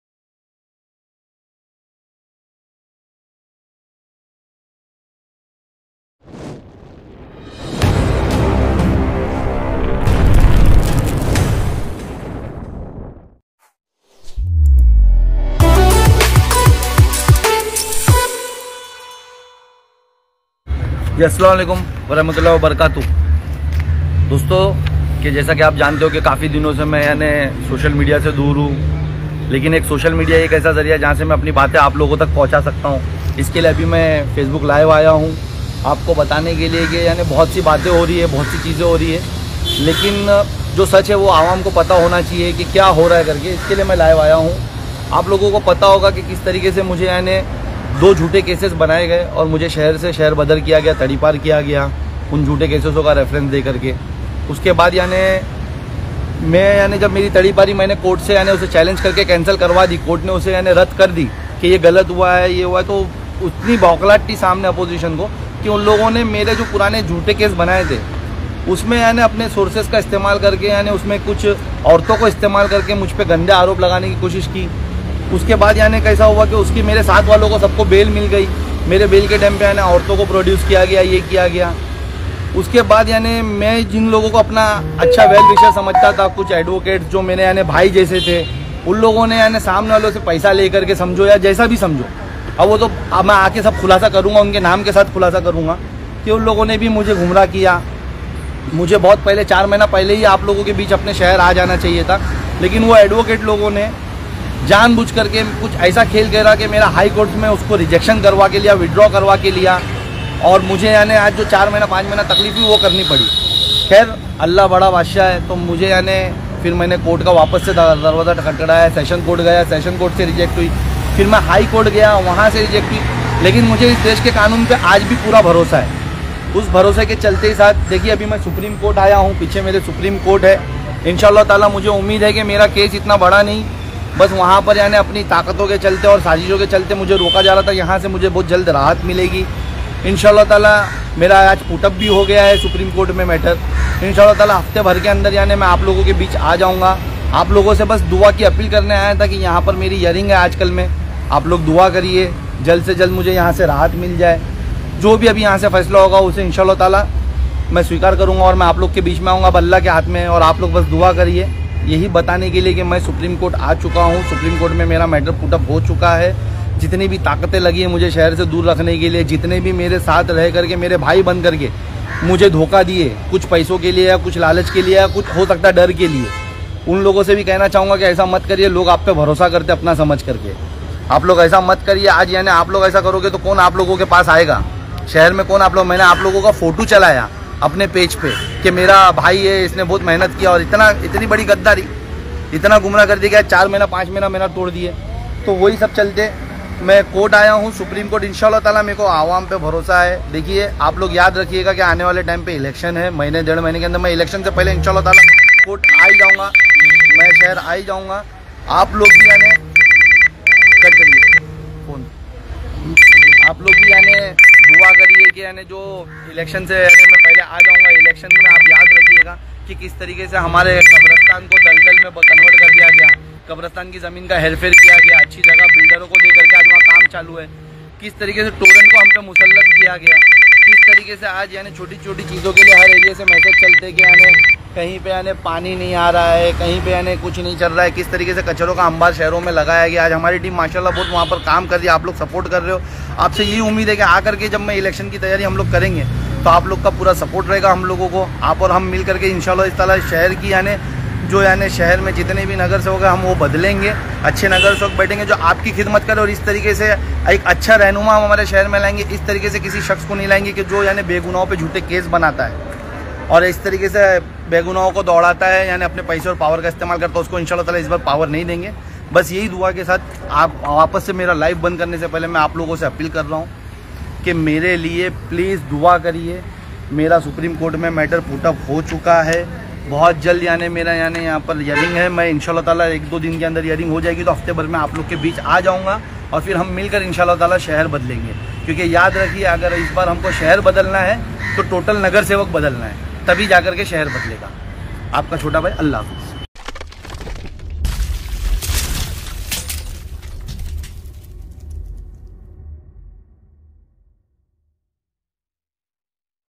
किस कारण नही आ पा रहे है सैफ पठान मुंब्रा , सोशल मीडिया पर किया खुलासा
एमआईएम कलवा मुंब्रा अध्यक्ष सैफ पठान ने सुप्रीम कोर्ट का दरवाजा खटखटाया , उन्होंने ने बताया के मेरे अपनो ने ही मुझे 5 से 6 महीने दूर कर दिया

MIM#mumbra#असदुद्दीनओवैसीNewsmumbra#